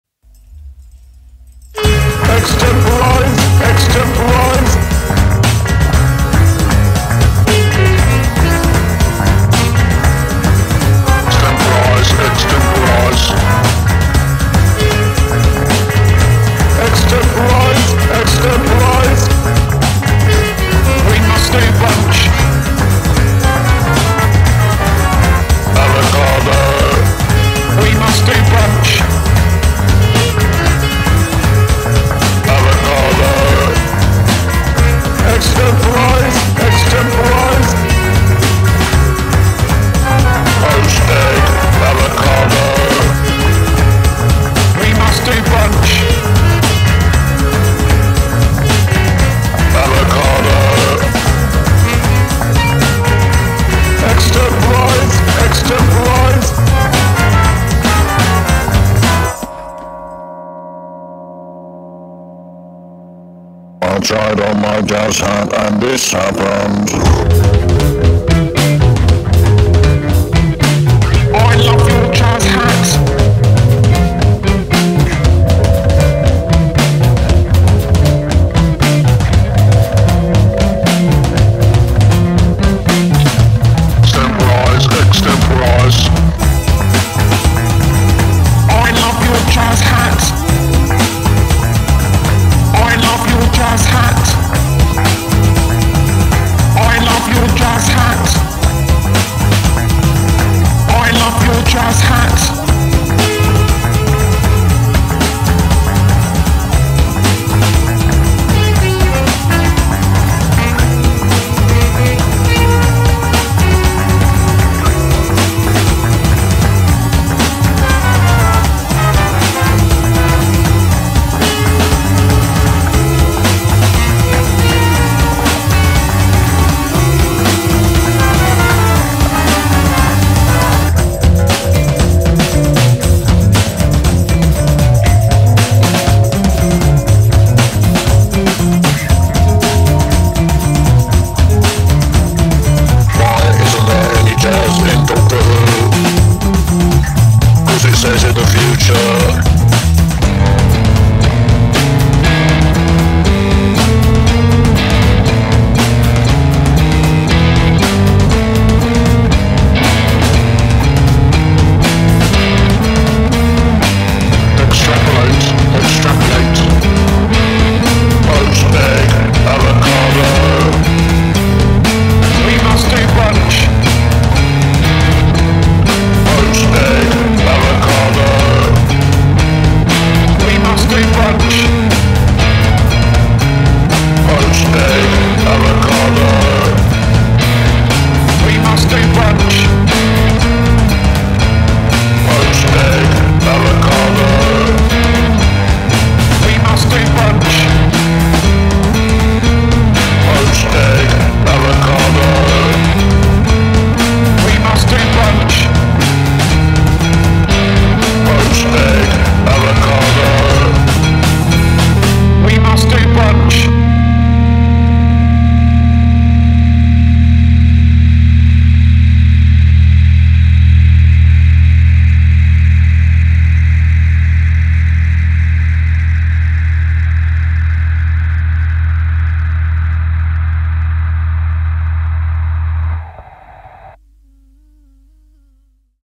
Dalek Jazz Apocalypse
What would happen if The Daleks got into jazz?

Welcome to the Dalek Jazz Apocalypse!

In collaboration with Molly, Matt, Crispin, Roger, and Nick from the @newnoteorchestra !